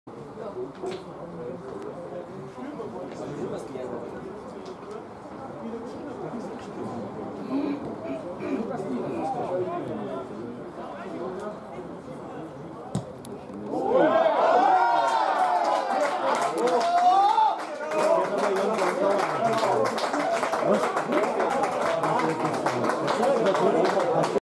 GL Nord #27 Tulln : Enzersfeld/W. - 0:1 (1. Halbzeit / 45:31) am 28.05.2016 17:48
GL Nord #27 Tulln : Enzersfeld/W.  - FC Tulln / SC Enzersfeld/W. - 0:1  (1. Halbzeit / 45:31) am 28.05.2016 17:48